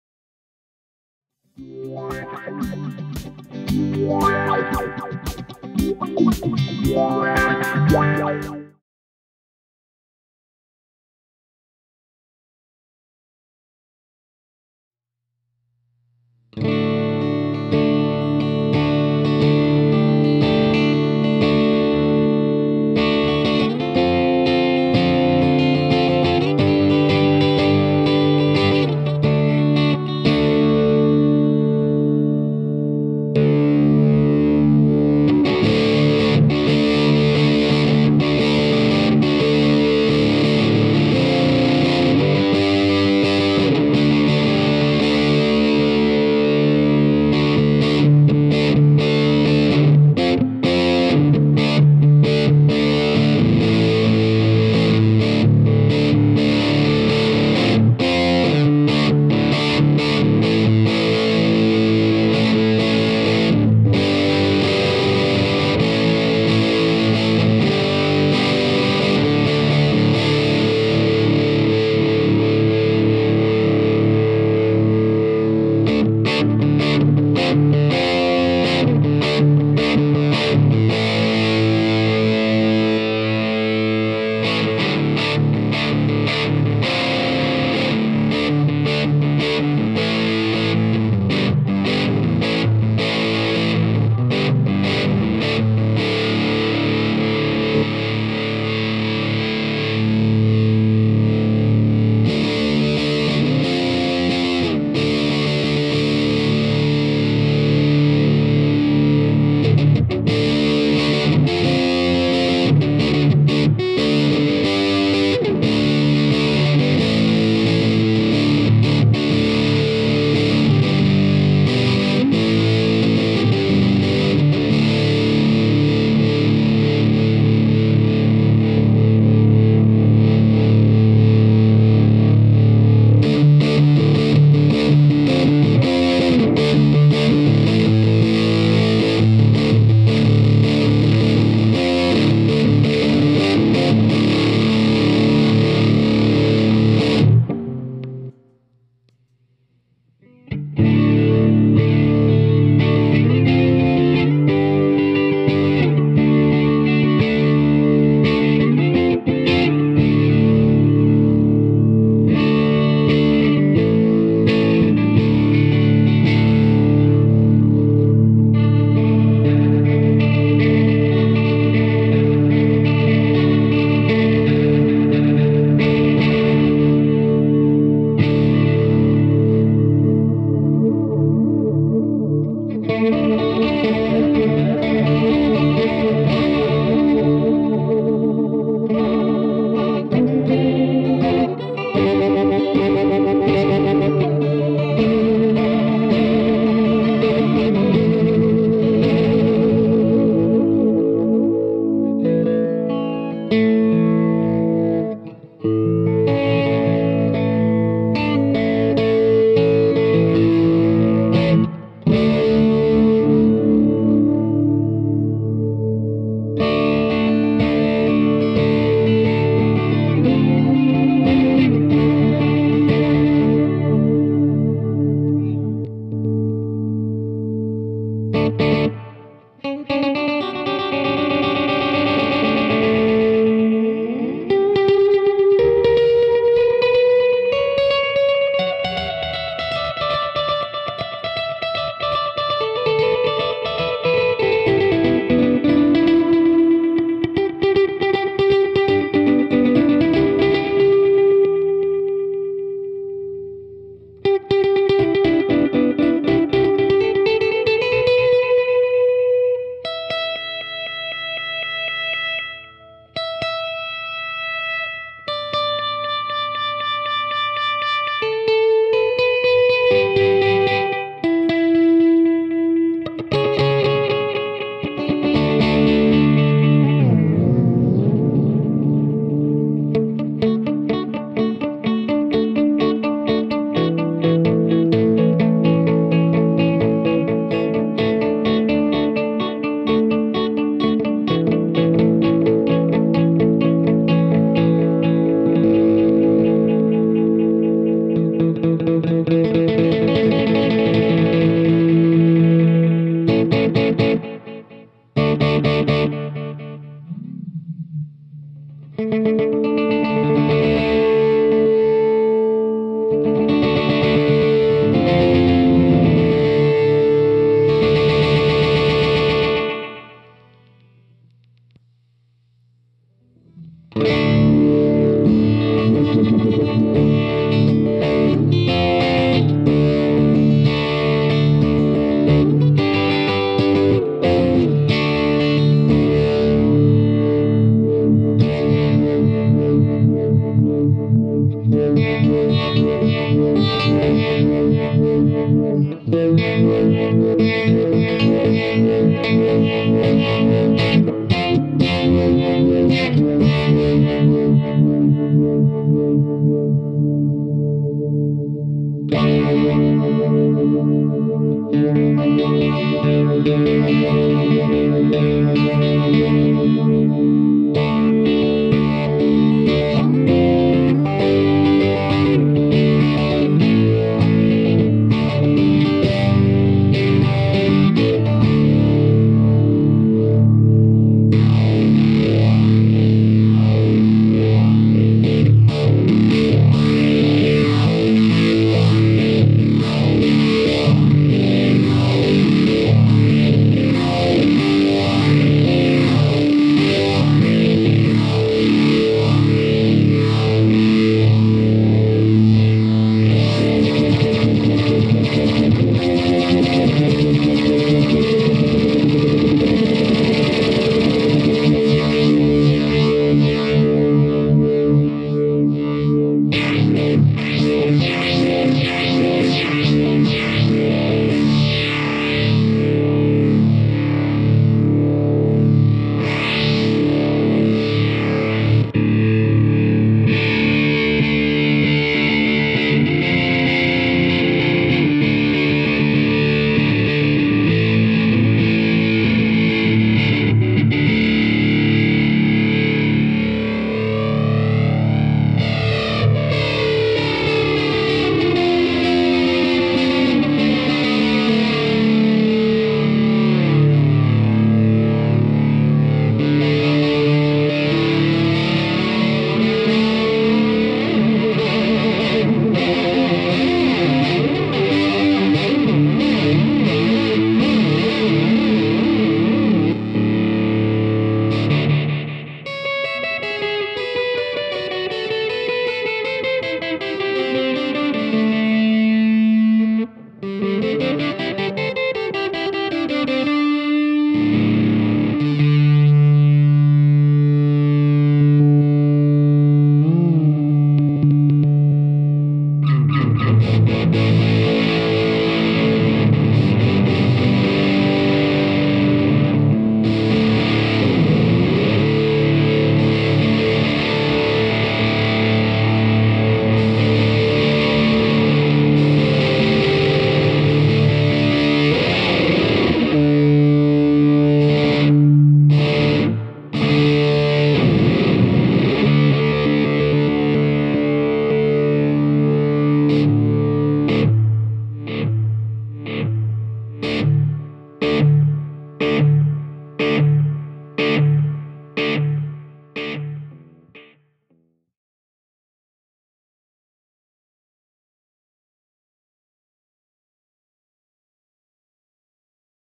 Keeley Electronics Dark Side Fuzz Delay Modulation Workstation
This is a demo video of the Dark Side Fuzz Delay Modulation Workstation effects pedal from Keeley Electronics performed and produced by Pedal of the Day. for more effects pedal reviews and demos, and be sure to check out more of the videos on our YouTube channel as well. Cheers!

Tone Tubby Purple Haze

sE Electronics & Shure Microphones

Spectraflex Cables